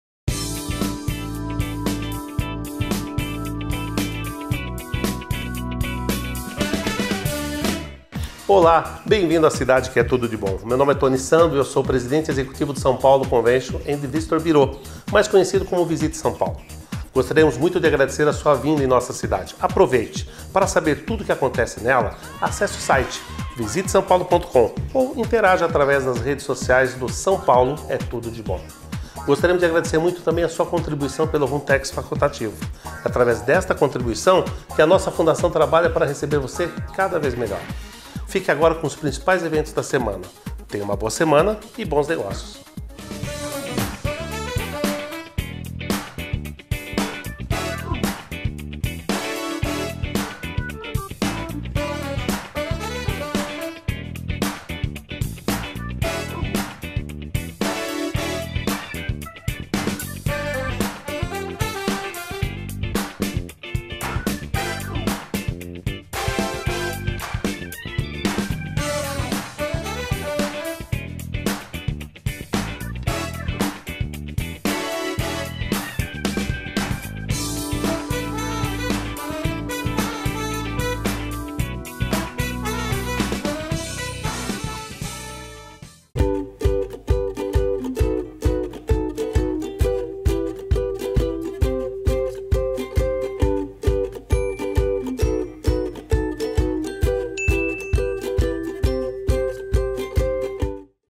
Agenda de São Paulo, 15 a 21 de outubro de 2018 - Com Toni Sando
Agenda de Eventos da cidade de São Paulo apresentada por Toni Sando, Presidente Executivo do VISITE SÃO PAULO. Facebook Twitter Linkedin